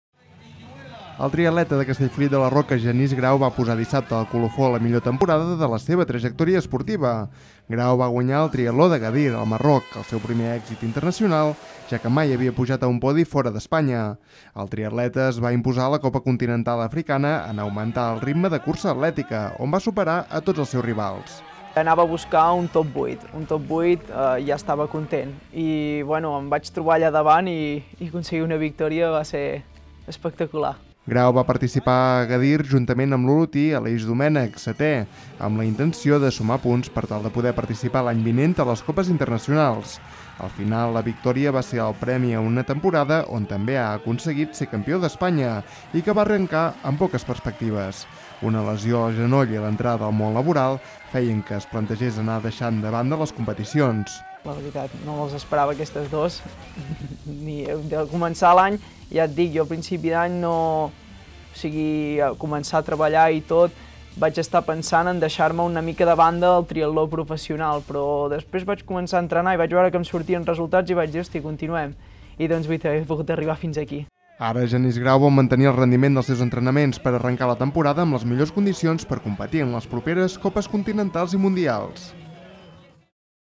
El triatleta Genís Grau completa una magnífica temporada
Genís Grau ha guanyat el primer títol internacional de la seva trajectòria. L’atleta de Castellfollit es va imposar dissabte al triatló d’Agadir, al Marroc, tancant d’aquesta manera la seva millor temporada, on també s’ha proclamat campió d’Espanya de l’especialitat.